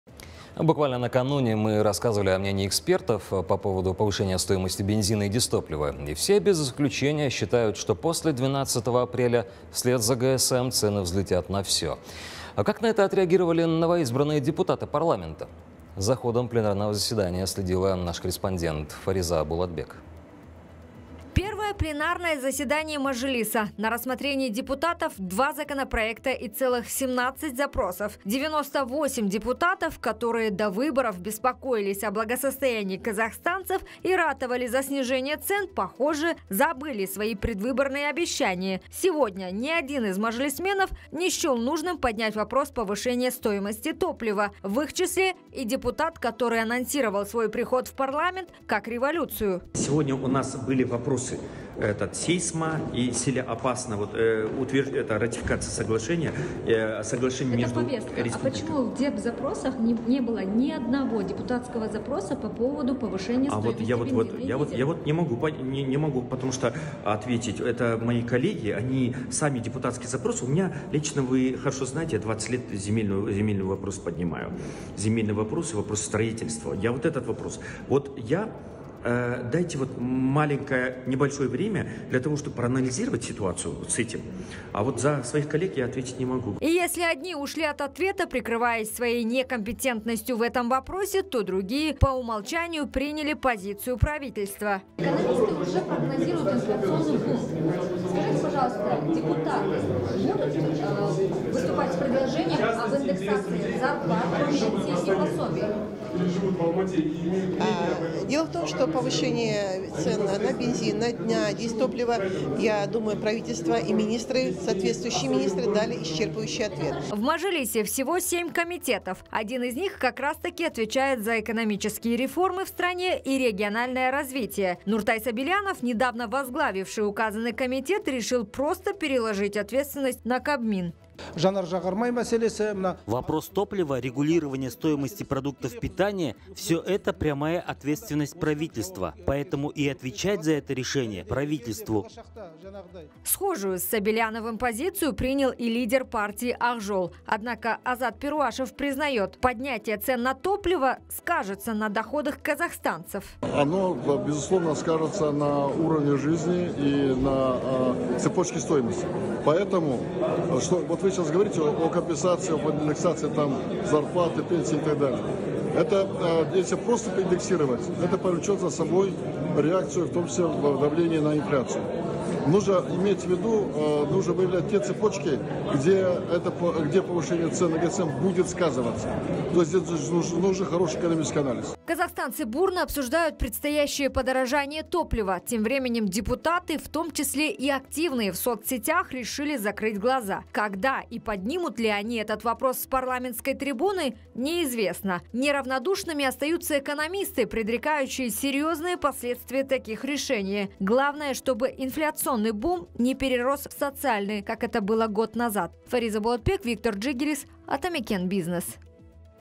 Как отреагировали депутаты на повышение стоимости ГСМ?
Мы и ранее рассказывали о мнении экспертов по поводу повышения стоимости бензина и дизтоплива. Все без исключения считают, что после 12 апреля, вслед за ГСМ, цены взлетят на всё. А как на это отреагировали новоизбранные депутаты Парламента? За ходом пленарного заседания следила наш корреспондент Фариза Болатбек